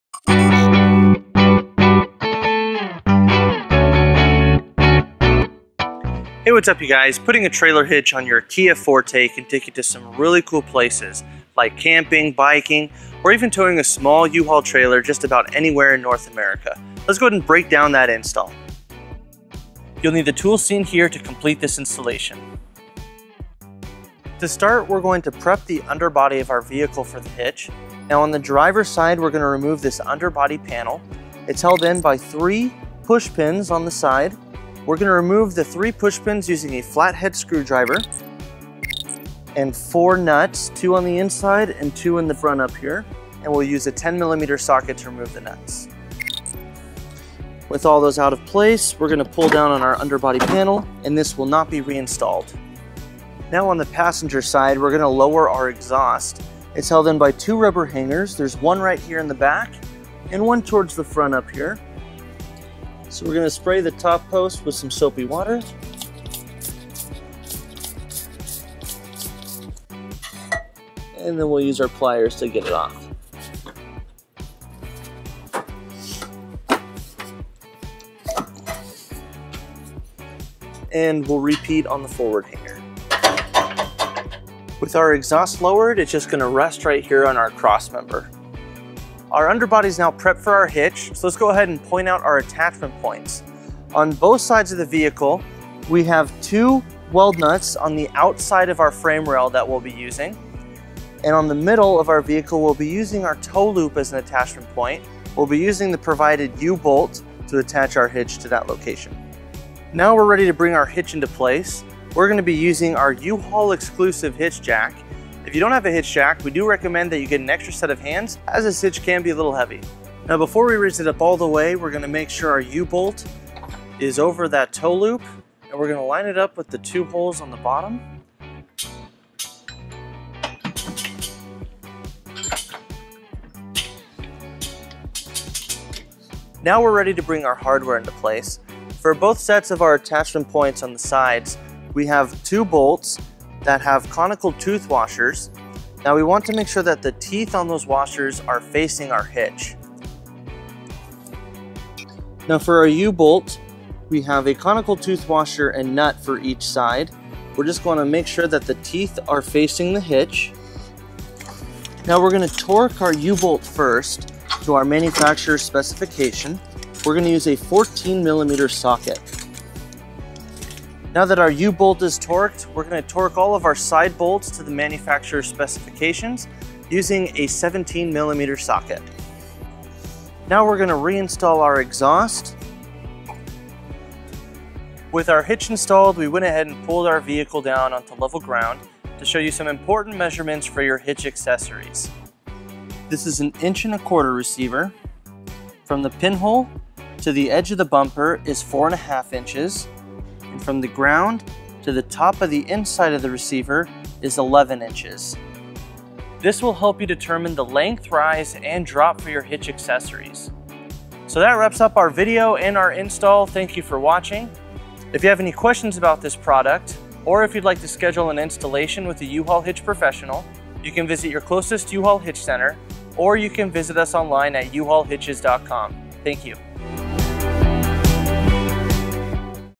2016 Kia Forte Trailer Hitch Installation
This video covers the trailer hitch installation for Curt 11368 on a 2016 Kia Forte. Learn how to install your receiver on your 2016 Kia Forte from a U-Haul installation professional. With a few tools and our step-by-step instructions, you can complete the installation of Curt 11368.

To purchase this trailer hitch and other towing accessories for your 2016 Kia Forte,

0:00 Intro
0:19 Tool list
0:24 Remove underbody panel
0:53 Lower exhaust
1:29 Raise hitch into place
2:16 Install hardware
2:41 Torque bolts
3:00 Reinstall exhaust
3:05 Hitch details and measurements
3:35 Outro